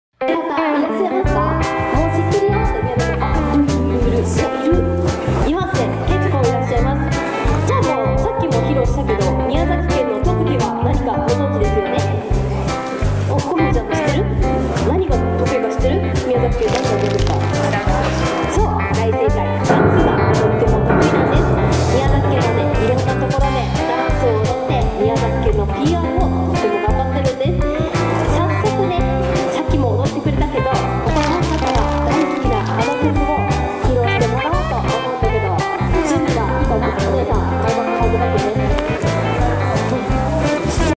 【みやざき犬】かぁくん、ちゃんとなおして！
これからダンスを行うので、三匹がもっているバッグを1つにまとめて片付ける むぅちゃん。 かぁくんは なかなか上手く「なおせない」。
「なおす」とは、宮崎弁で「かたずける」の意味。
【例】なおしちょって！＝片付けて